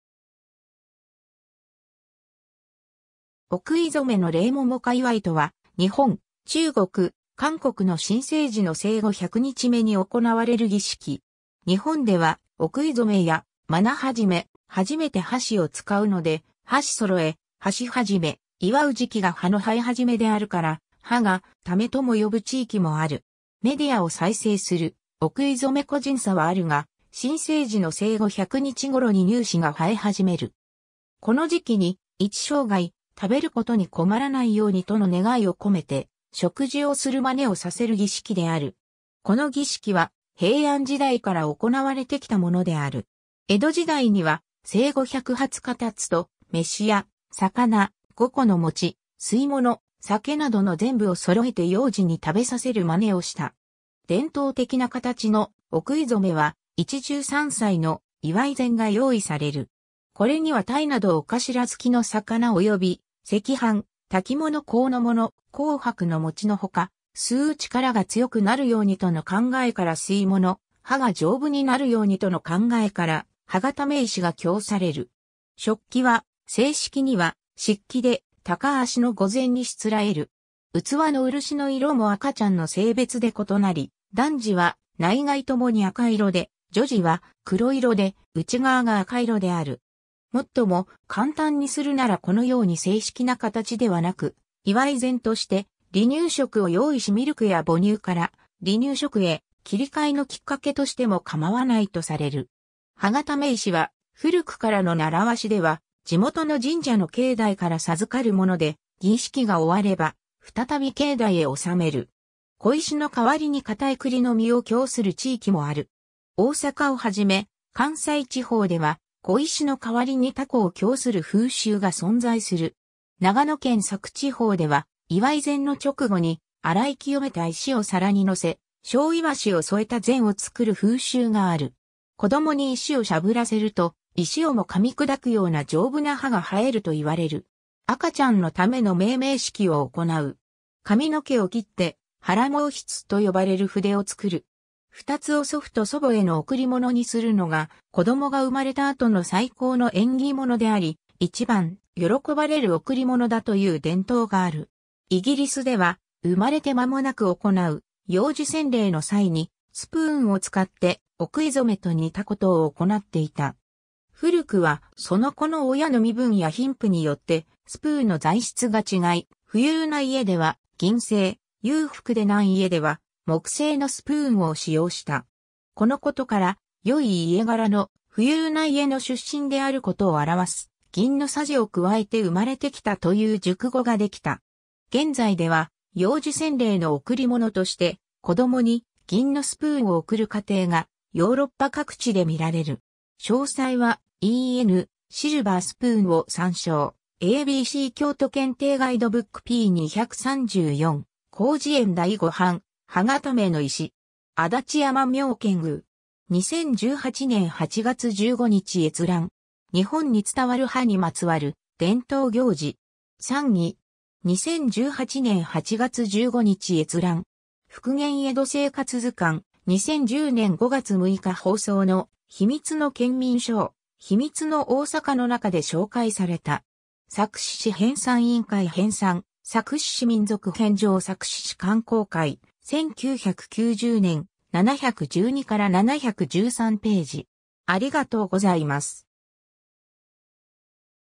百日祝い
お食い初めの例 百日祝い（ももかいわい）とは、日本、中国、韓国の新生児の生後100日目（または110日目、120日目）に行われる儀式。
日本では「お食い初め（おくいぞめ）」や「真魚始め（まなはじめ）」、初めて箸を使うので「箸揃え（はしそろえ）」「箸初め（はしぞめ）」、祝う時期が歯の生え始めであるから「歯がため」とも呼ぶ地域もある。
 メディアを再生する お食い初め 個人差はあるが、新生児の生後100日頃に乳歯が生え始める。
この時期に「一生涯、食べることに困らないように」との願いを込めて食事をする真似をさせる儀式である。
 この儀式は、平安時代から行われてきたものである。
 江戸時代には、生後120日経つと、飯や魚、5個の餅、吸い物、酒などの膳部（ぜんぶ：膳にのせて供する食物・料理）をそろえて幼児に食べさせる真似をした。
 伝統的な形の「お食い初め」は、一汁三菜の「祝い膳（いわいぜん）」が用意される。
これには鯛など尾頭（おかしら）つきの魚および、赤飯・焚き物・香の物・紅白の餅のほか、吸う力が強くなるようにとの考えから吸い物（汁物）、歯が丈夫になるようにとの考えから歯固め石が供される。
食器は、正式には漆器で高足の御膳にしつらえる。
器の漆の色も赤ちゃんの性別で異なり、男児は内外ともに赤色で、女児は黒色で内側が赤色である。
もっとも、簡単にするならこのように正式な形ではなく、祝い膳として離乳食を用意しミルクや母乳から離乳食へ切り替えのきっかけとしても構わないとされる。
 歯固め石は、古くからの習わしでは地元の神社の境内から授かるもので、儀式が終われば再び境内へ納める。
小石の代わりに固いクリの実を供する地域もある。
大阪をはじめ、関西地方では小石の代わりにタコを供する風習が存在する。
 長野県佐久地方では、祝い膳の直後に、洗い清めた石を皿に乗せ、焼鰯を添えた膳を作る風習がある。
子供に石をしゃぶらせると、石をも噛み砕くような丈夫な歯が生えるといわれる。
 赤ちゃんのための命名式を行う。
 髪の毛を切って「胎毛筆」と呼ばれる筆を作る。
二つを祖父と祖母への贈り物にするのが、子供が生まれた後の最高の縁起物であり、一番喜ばれる贈り物だという伝統がある。
 イギリスでは、生まれて間も無く行う幼児洗礼（命名式）の際に、スプーンを使ってお食い初めと似たことを行っていた。
古くは、その子の親の身分や貧富によってスプーンの材質が違い、富裕な家では銀製、裕福でない家では木製のスプーンを使用した。
このことから、良い家柄の・富裕な家の出身であることを表す「銀の匙をくわえて生まれてきた」という熟語ができた。
現在では、幼児洗礼の贈り物として子供に銀のスプーンを贈る家庭がヨーロッパ各地で見られる。
 詳細は「en:Silver spoon」を参照 ^ a b c 京都検定ガイドブック p.234 ^ 広辞苑第5版 ^ “歯固めの石（お食い初め、百日のお祝い）”. 足立山妙見宮. 2018年8月15日閲覧。
 ^ “日本に伝わる歯にまつわる伝統行事”. サンギ. 2018年8月15日閲覧。
 ^ 『復元　江戸生活図鑑』（笹間良彦著、柏書房、113ページ） ^ 2010年5月6日放送の『秘密のケンミンSHOW』「ヒミツのOSAKA」の中で紹介された。
 ^ 佐久市志編纂委員会編纂『佐久市志 民俗編 上』佐久市志刊行会、1990年、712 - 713ページ。